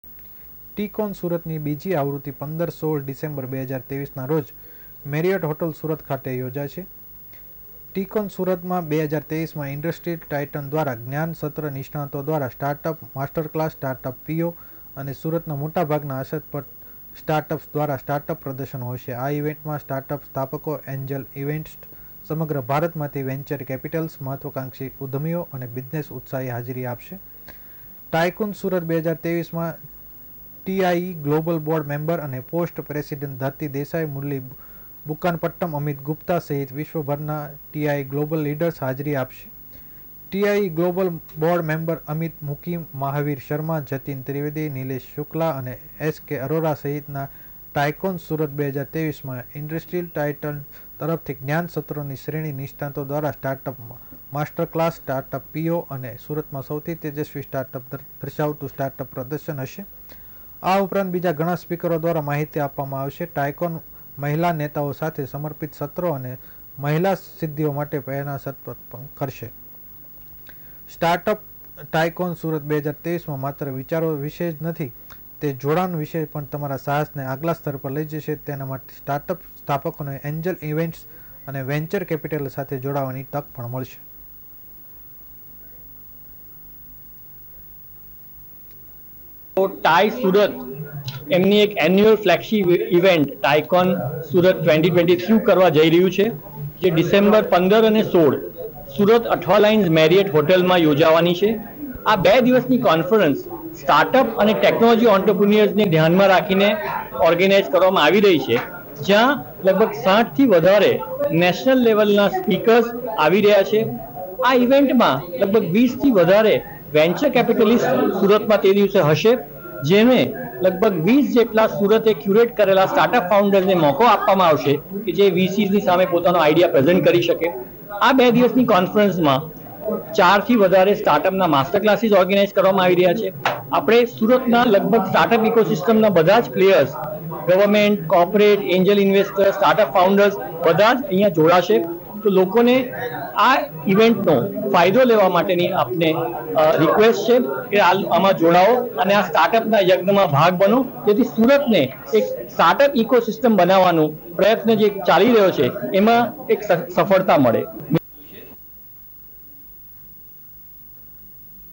TiECon સુરતની 2જી આવૃત્તિ 15-16 ડિસેમ્બર, 2023ના રોજ મેરિયોટ હોટેલ સુરત ખાતે યોજાશે...
The 2nd edition of TiECon Surat will be held on December 15-16, 2023 at Marriott Hotel Surat...